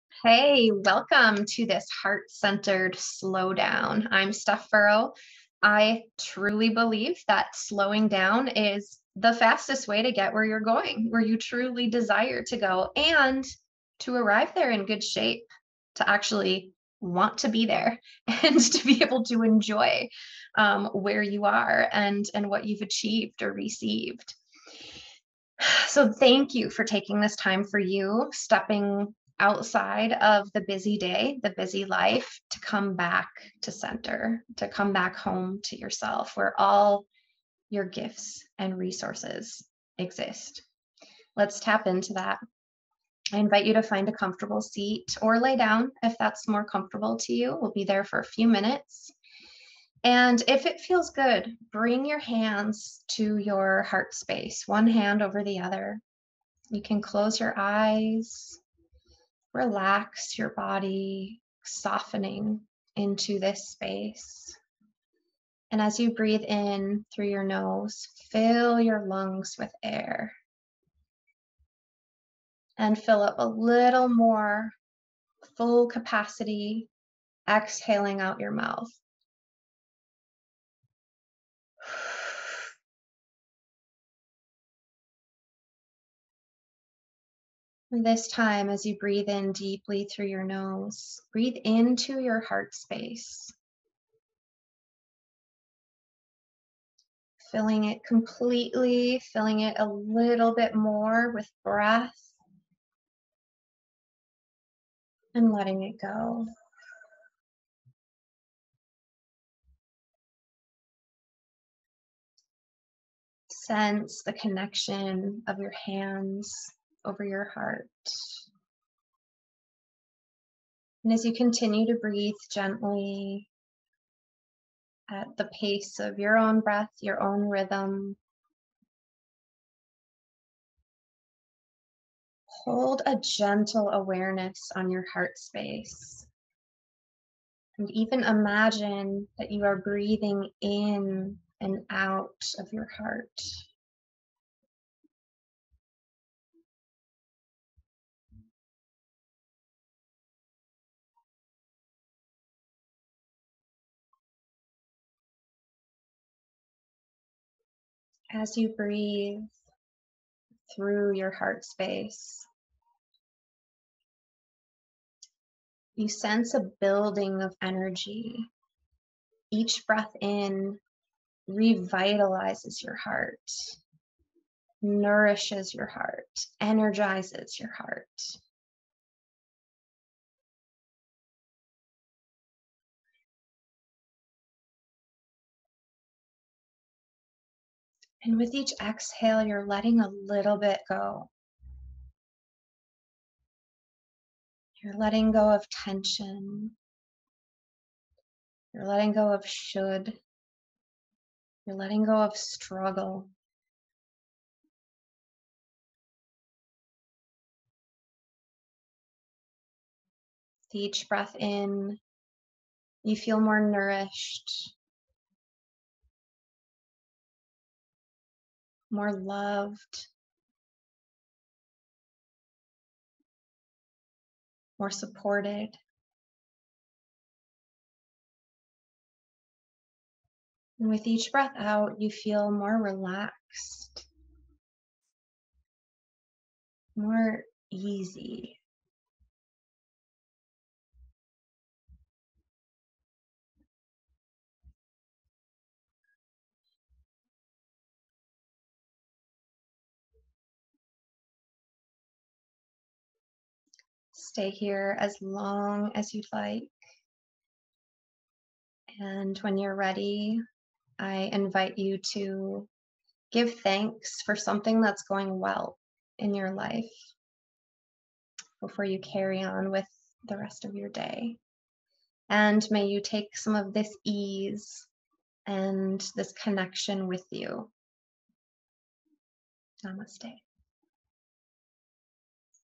🐢 SLOW DOWN - Heart Breath
It's the tortoise who wins the race, remember?

Your power isn't in what you do, it rests in who you are.
Slow down, come back to yourself, and move with more ease.

Join me for some beautifully grounding heart breathing 💖